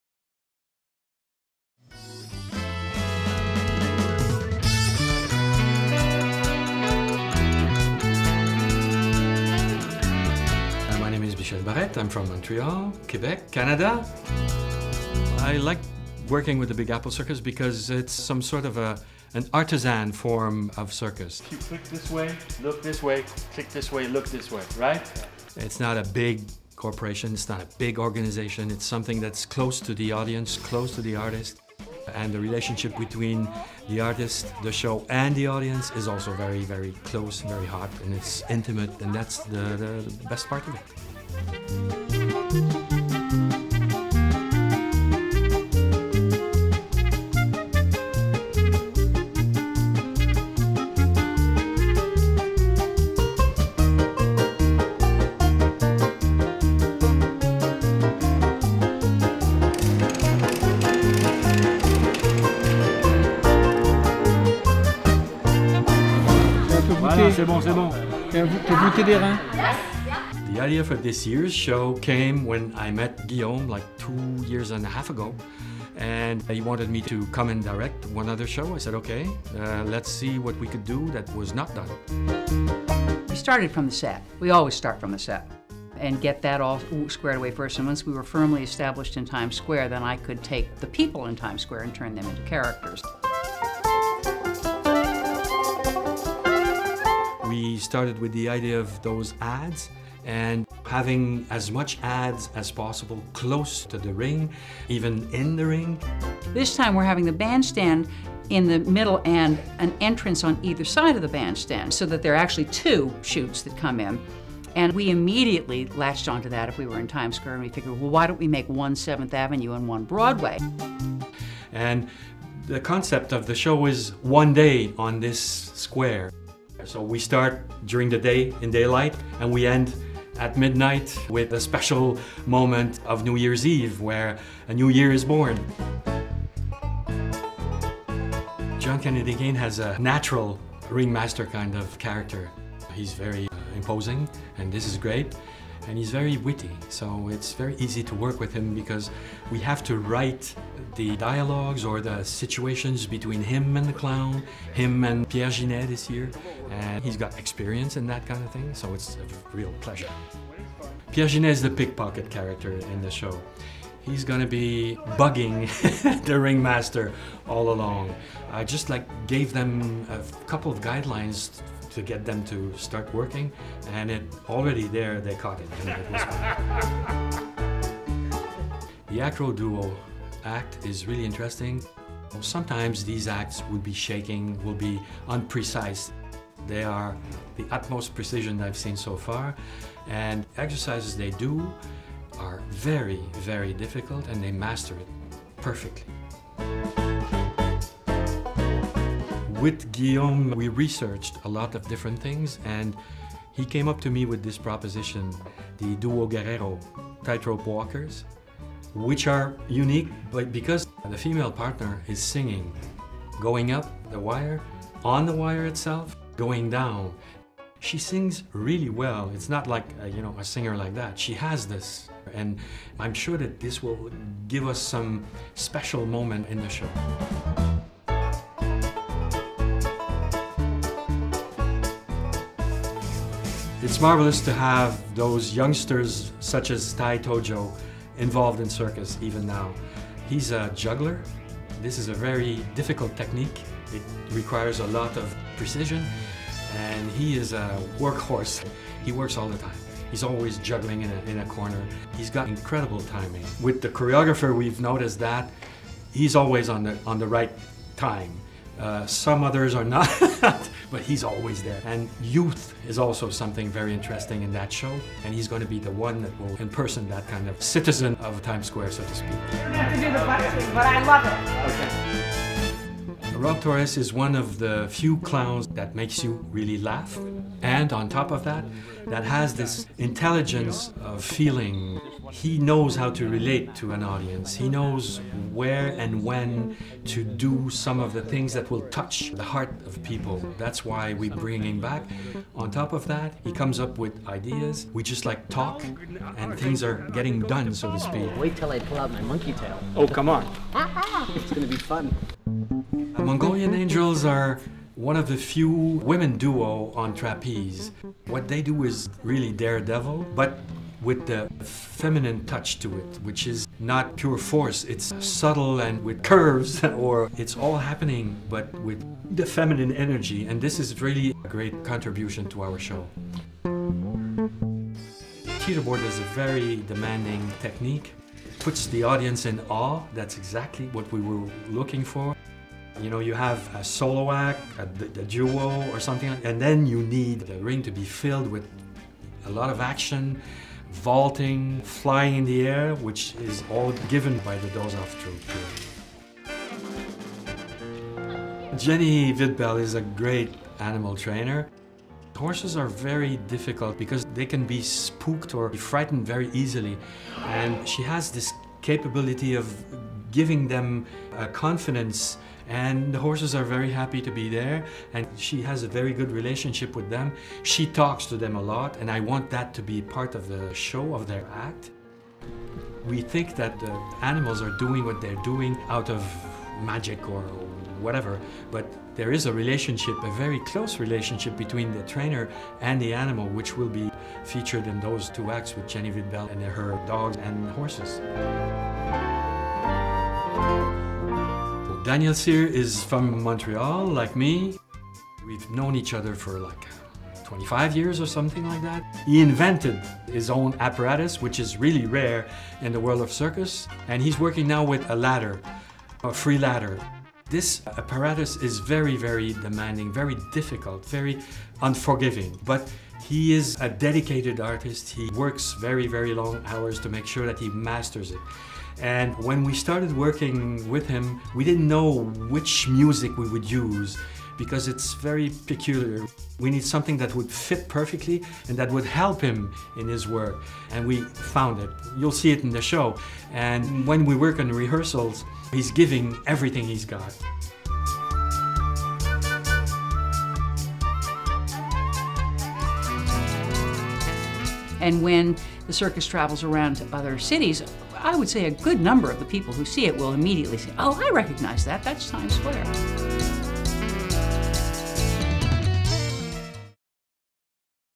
Big Apple Circus Lincoln Center Tent Video 2013
Video for tent at Lincoln Center of Big Apple Circus Artists preparing for their 36th season and the new show 'LUMINOCITY". Produced, shot and edited by John Miglietta of JDM Productions, Inc. 2nd Camera: Richard Trammell.